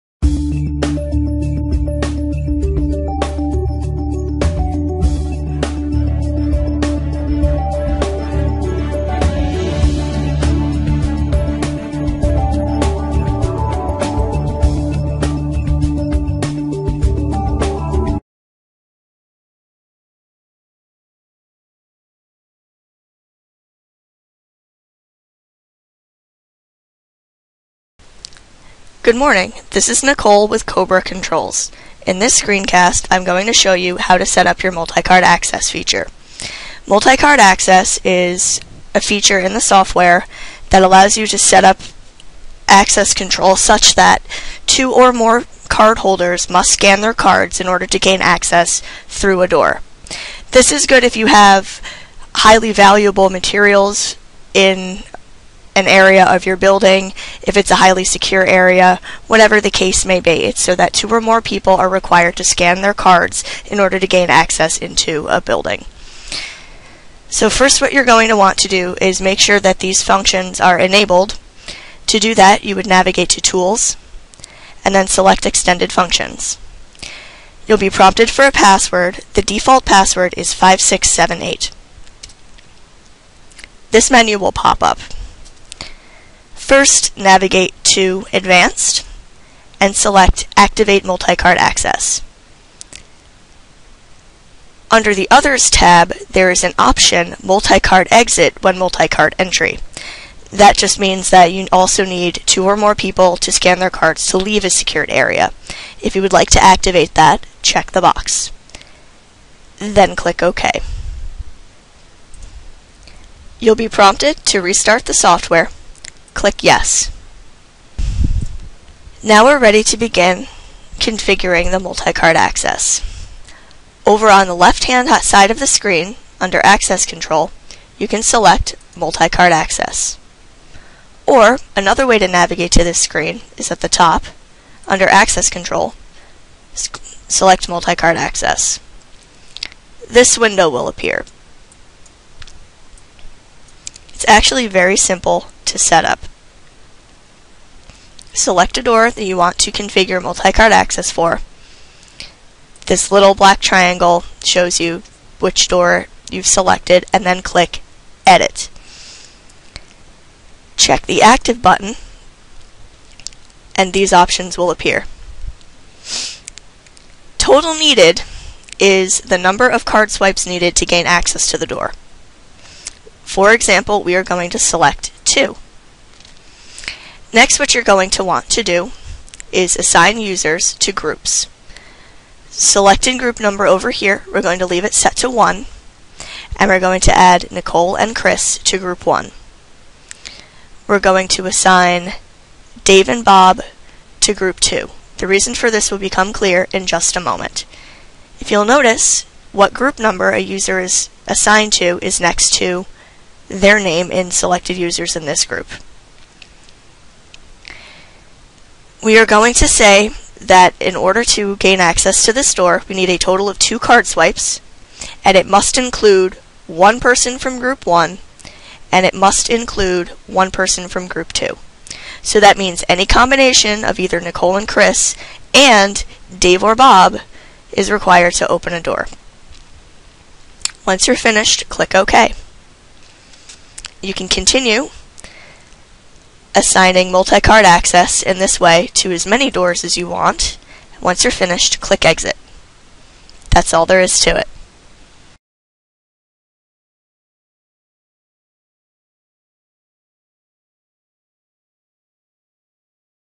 How to program Multi-Card Access - Cobra Controls Video Training Series
Cobra Controls Video Training Series - How to program Multi-Card Access using the Cobra Controls Door Access Control Software.